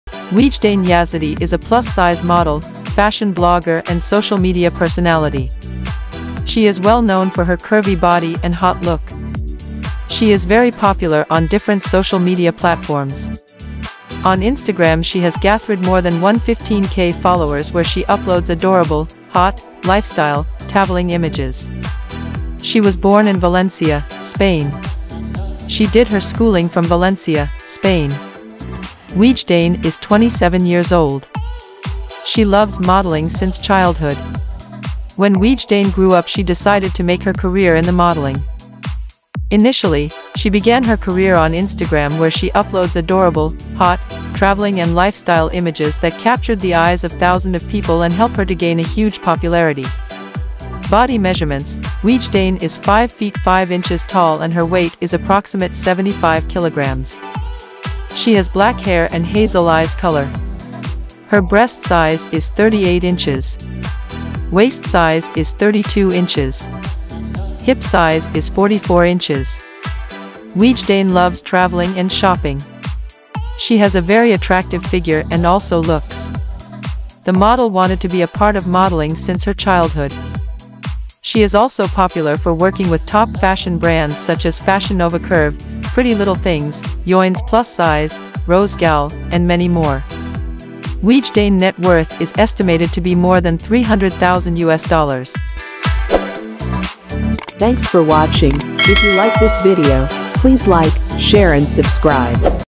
Ouijdane Yazidi Biography | Lifestyle | Body Measurements | Career | Facts | Boyfriend | Plussize
Ouijdane Yazidi is a plus-size model, digital content creator, and social media personality. Ouijdane Yazidi is well known for her curvy body and attractive looks. She is very popular on different social media platforms.

Here you will get to know all about plus-size model Ouijdane Yazidi Biography, Lifestyle, career, Net Worth, Family, Body Measurements, Age, Weight, Figure, and many interesting facts you can find here.

0:00 - Ouijdane Yazidi Biography
0:22 - Ouijdane Yazidi Birth Place
0:29 - Ouijdane Yazidi Age
0:51 - Ouijdane Yazidi Body Measurements
1:31 - Ouijdane Yazidi Net Worth